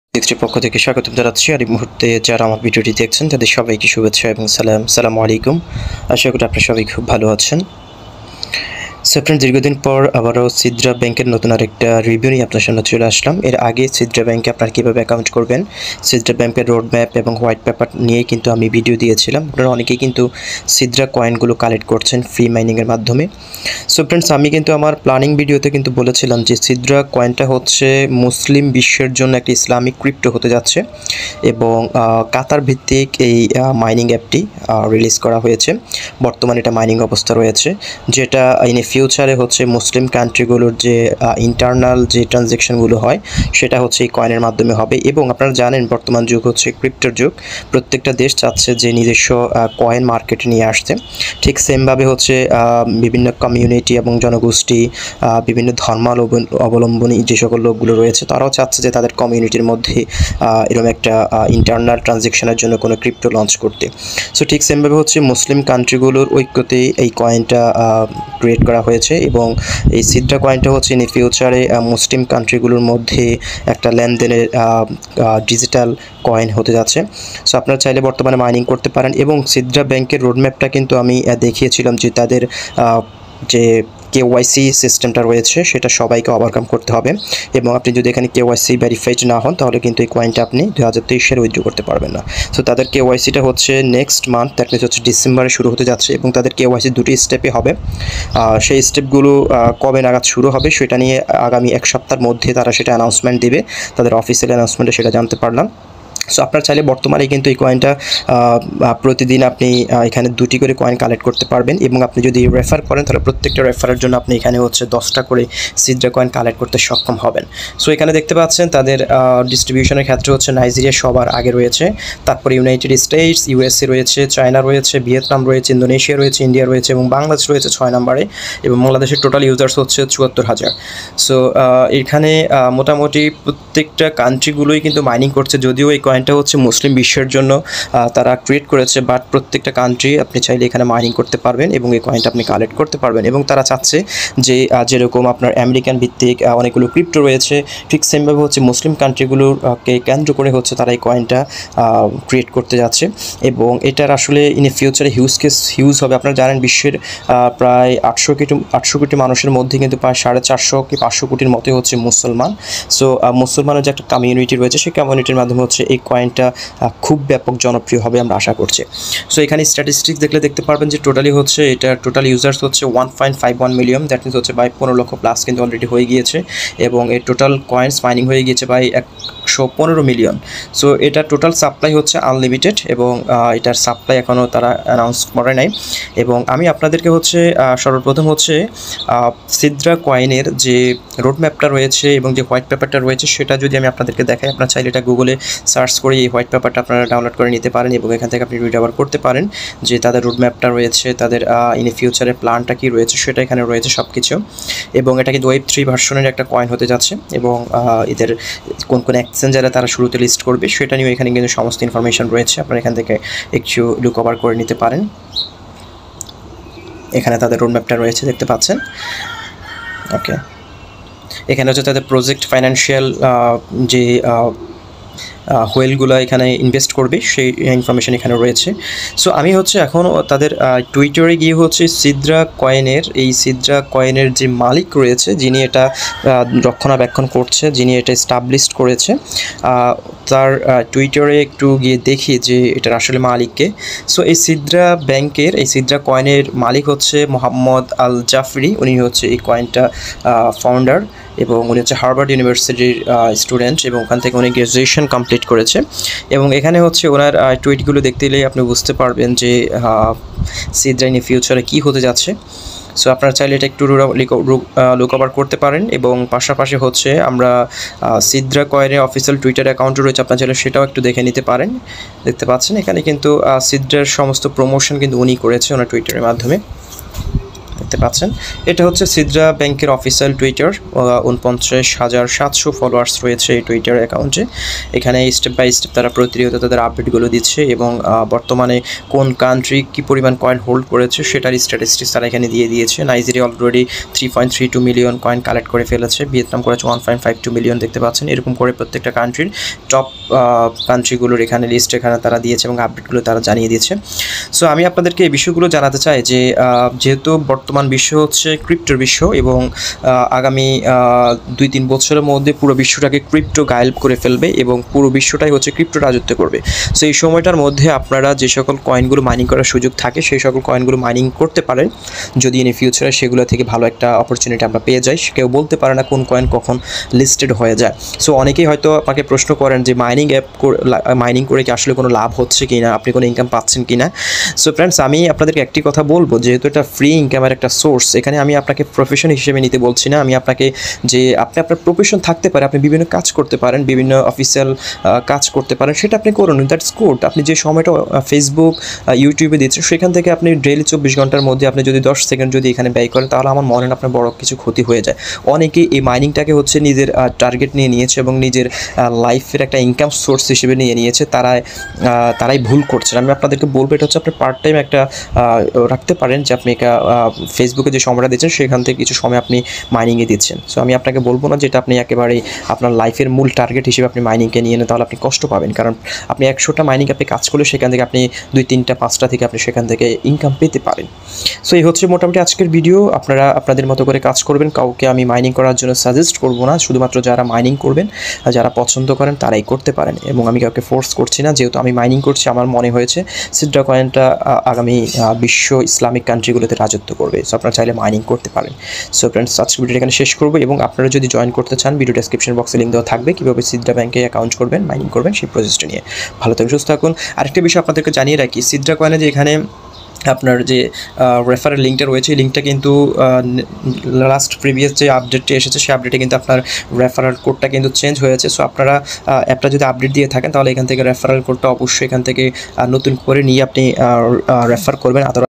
SIDRA Coin KYC Will Happend Two Phases | KYC will Start December @Bongo Tech360
BTCs Chinese very strong project, This coin will be the nearest value of Bitcoin.so feel free to Collect free BTCs Coin..

কোন সাইট, এপ প্রমোশন করতে চাইলে যোগাযোগ করতে পারেন.

thanks for Valuable Time